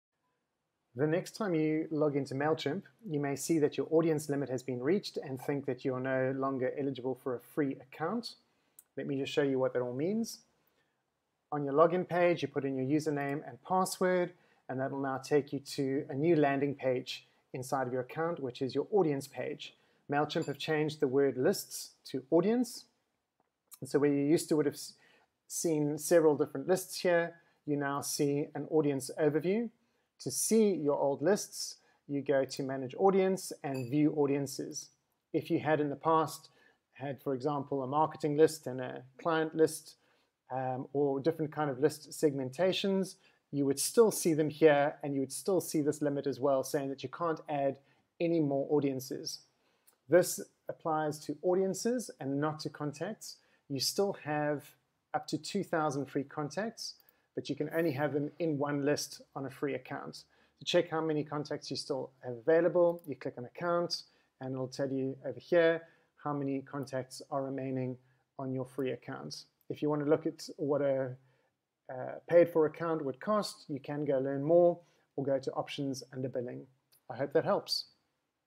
"MailChimp Audience Limit Reached" - here's what it actually means
A lot of our clients are logging into their MailChimp accounts to add contacts and thinking that they have to upgrade to a paid for account. They don't. Watch the video to find out why.